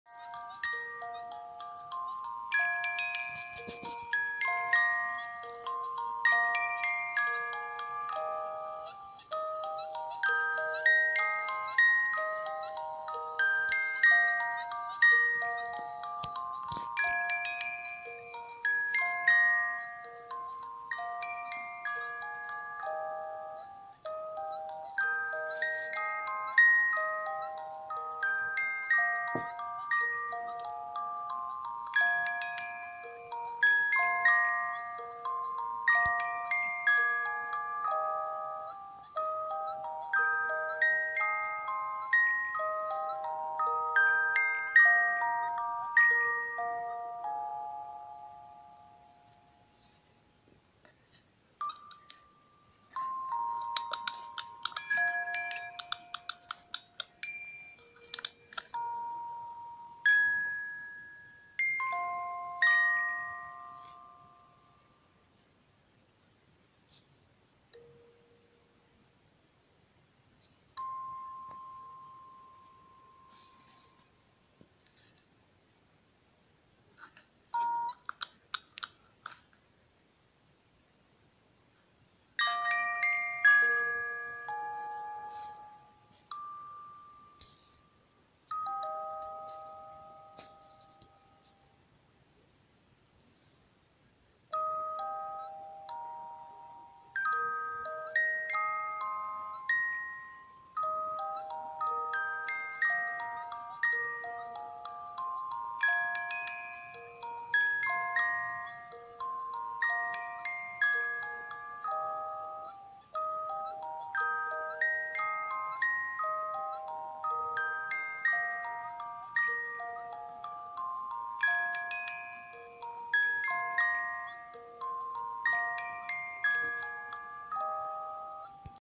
Carousel Tune: currently unknown, looking for info.
I don't know what the tune is despite my research.

Please let me know what the tune is.